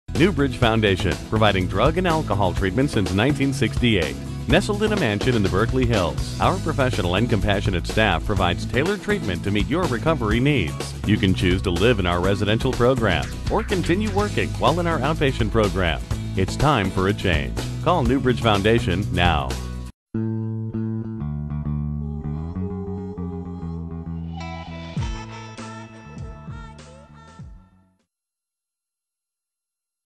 Berkeley Rehab Centers New Bridge Foundation
Need help overcoming an addiction? New Bridge Foundation in Berkeley, California, has over 40 years experience helping clients successfully end their dependence on drugs or drink through residential programs, outpatient treatment, counseling, youth education services and more. Let them help you help yourself.